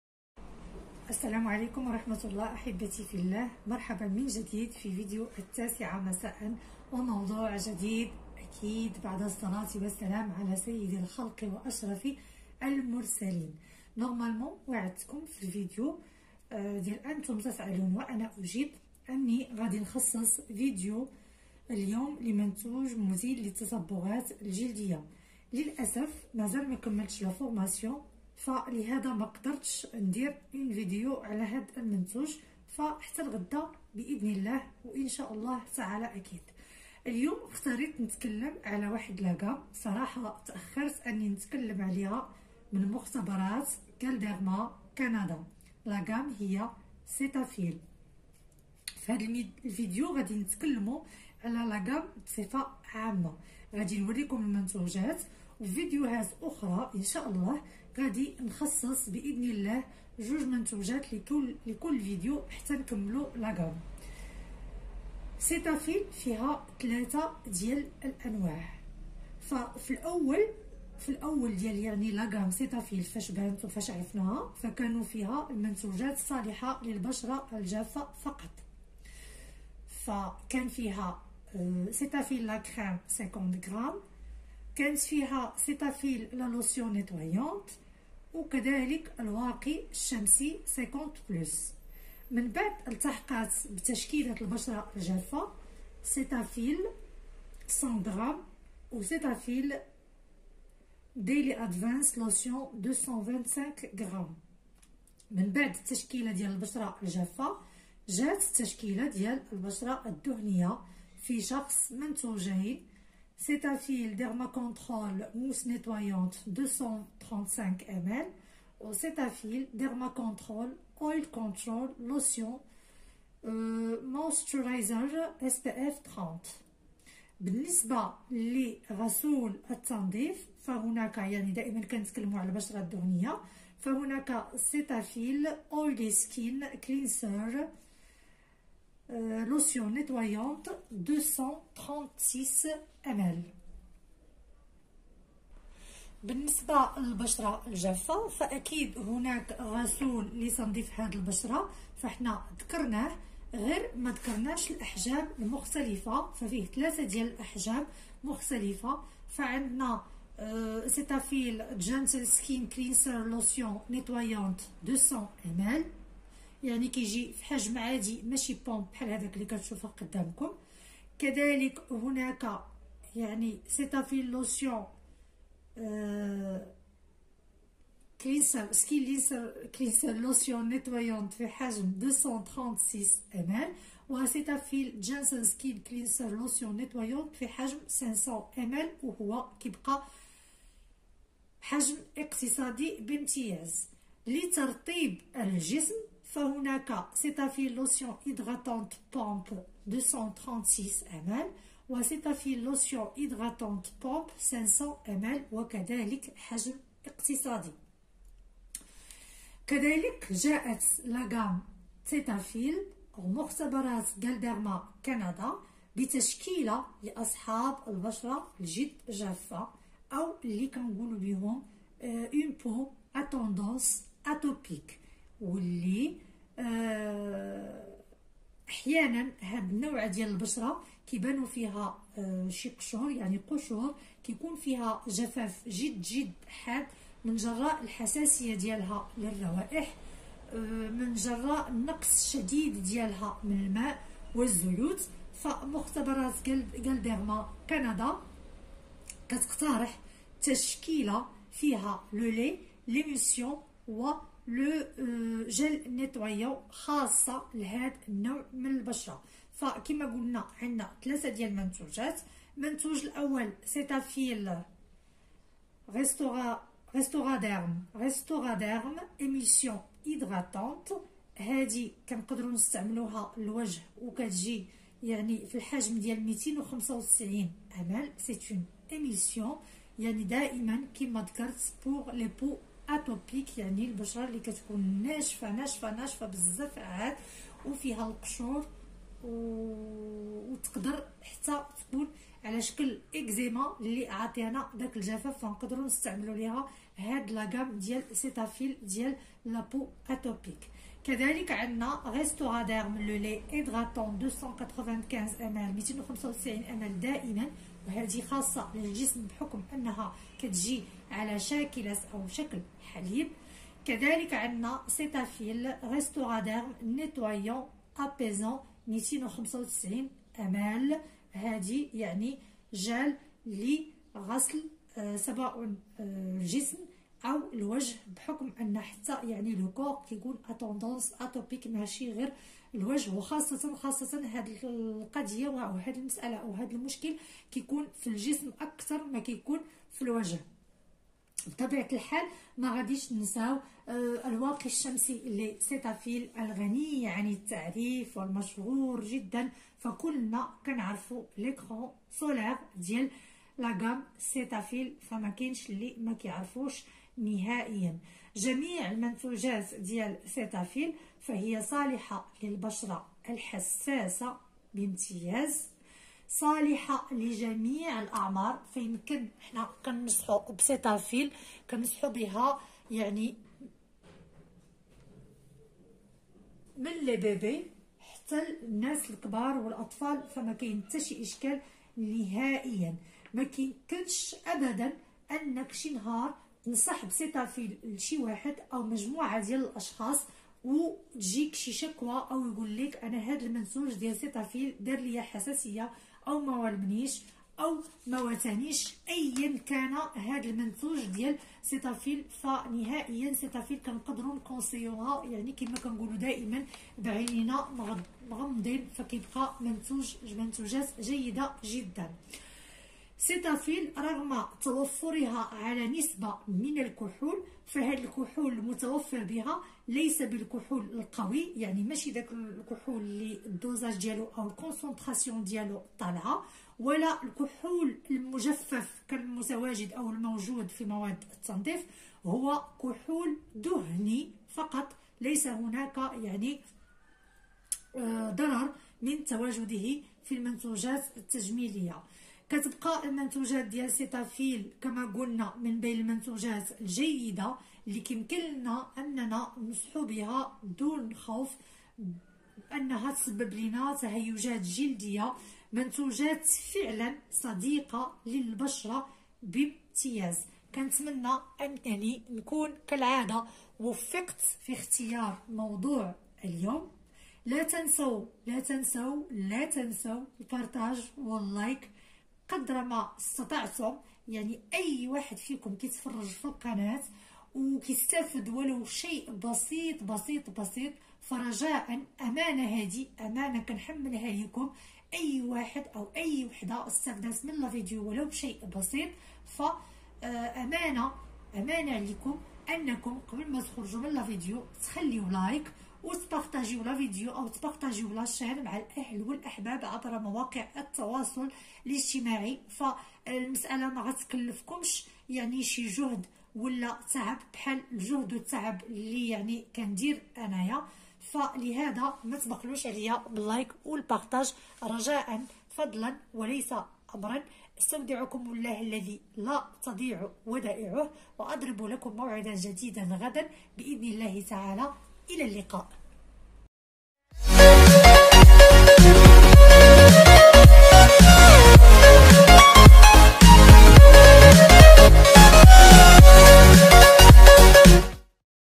Cetaphil Galderma Canadaتشكيلة من المنتوجات لجميع انواع البشرة
coin beaute
أكد من اختيار منتج ترطيب الوجه المناسب لنوع البشرة التي تمتلكها
لا تنسي وضع مرطب الوجه قبل المكياج دائماً
لا تعرض بشرتك للماء الساخن لوقت طويل أثناء الاستحمام
يجب وضع مرطب للوجه بعد الاستحمام دائما
شرب كمية مناسبة من الماء يعد المفتاح الأساسي لترطيب الوجه والحفاظ على نضارة البشرة
ترطيب البشرة